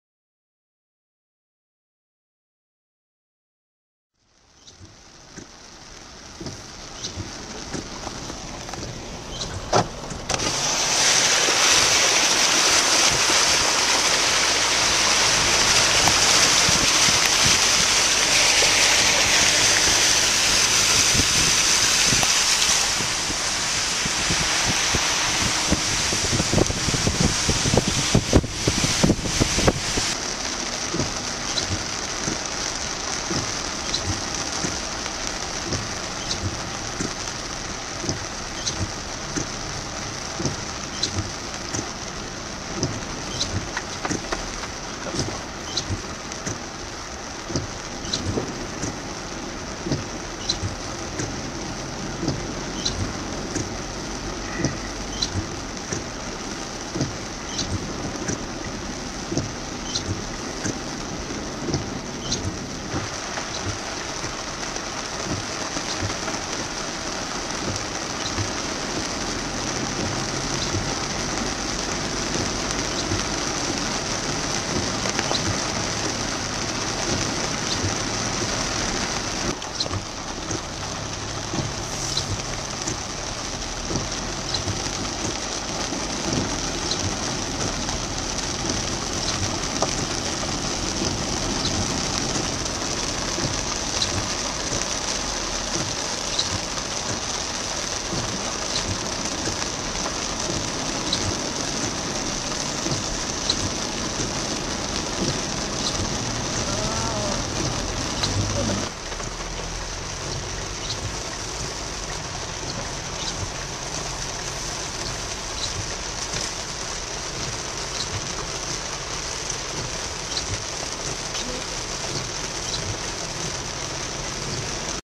Carolina Beach, NC Rain & Flood August 1, 2012
Some flooding at the Carolina Beach Lake area and the Central Business District. August 1, 2012 at 9:45am.